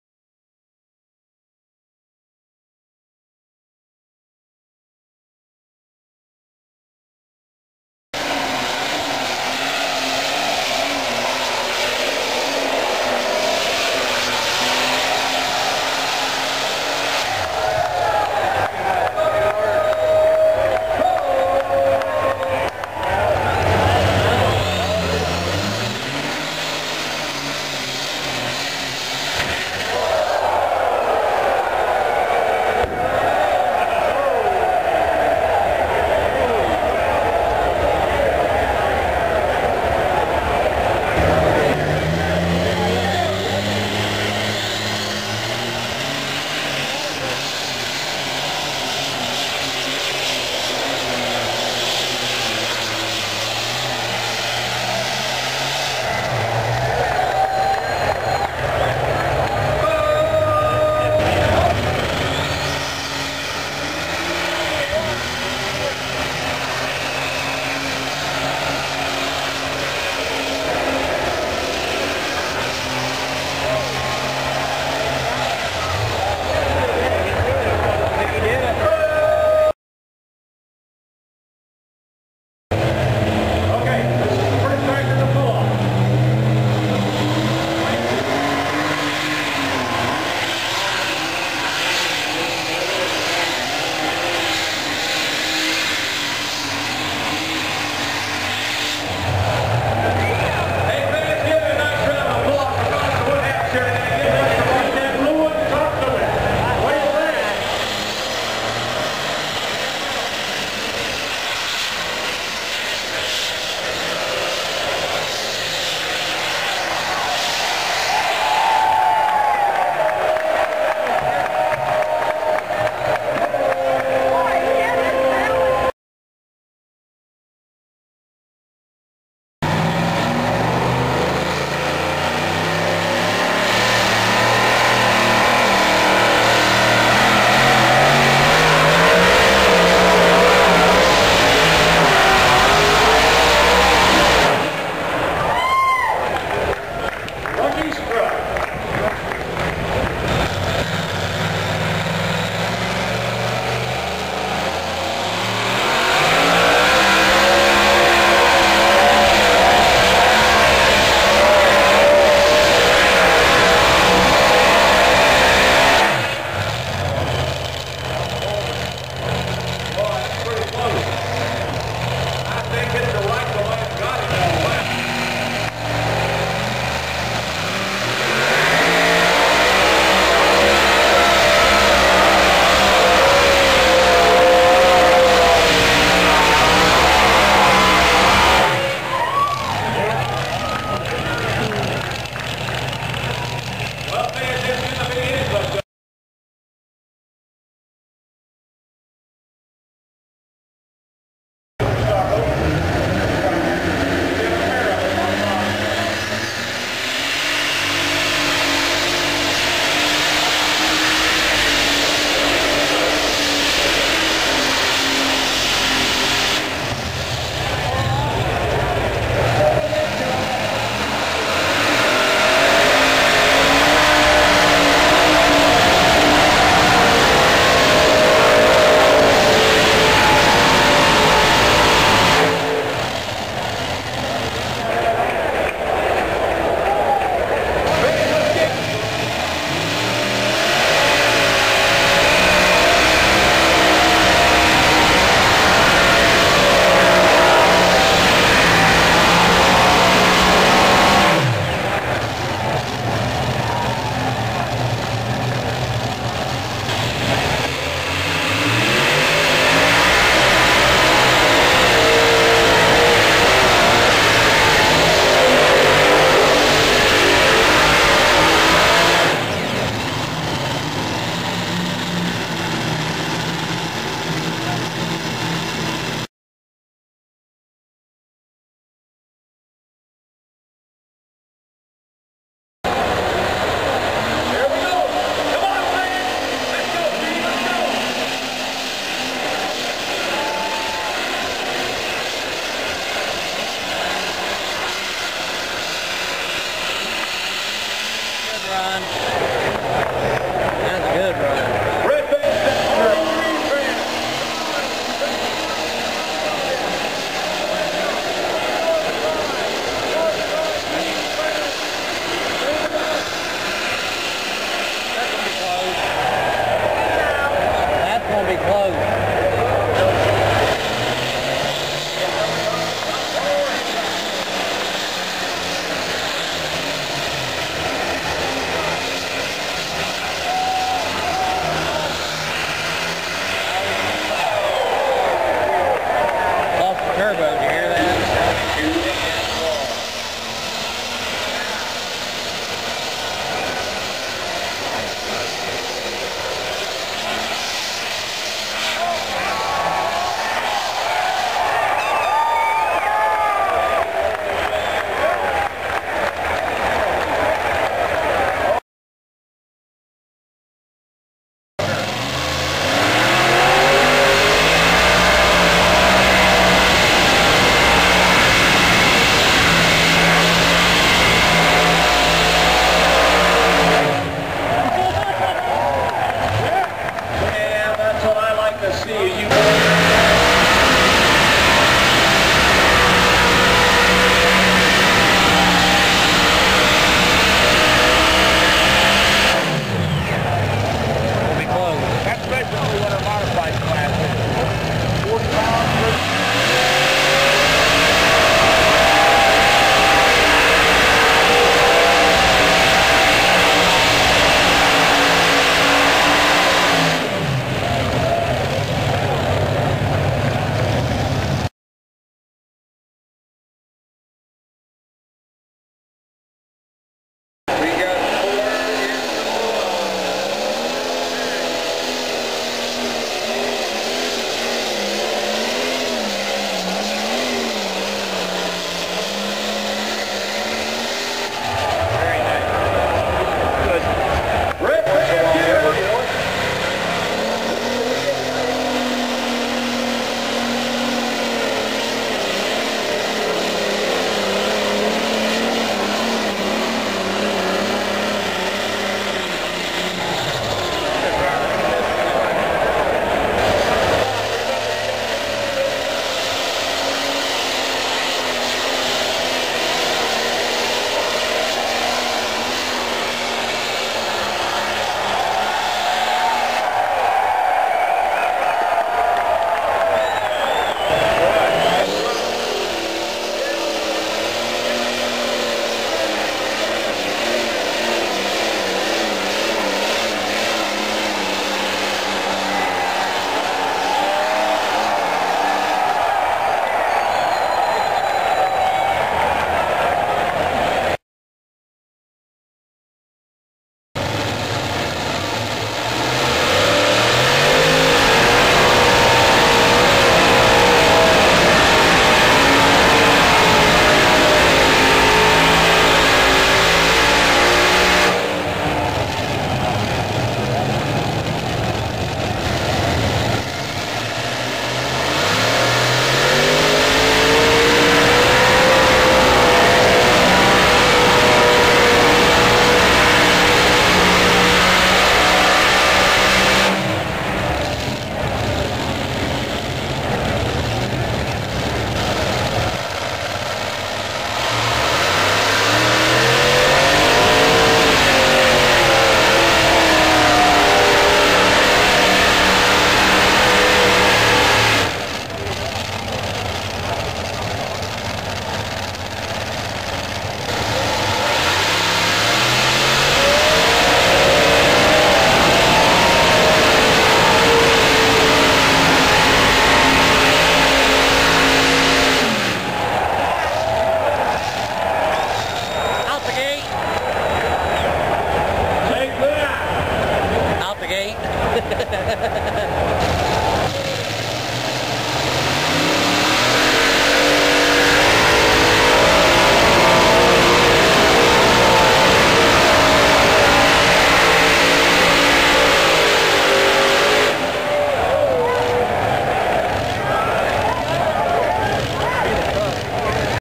NTPA: Bowling Green, Ohio highlights. 2008
Pulloffs from the National Tractor Pulling Championship
Session 1
SSO
TWD
LTSS

Session 2
Mods

Session 3
SSD
Mods

Session 4
PS
UNL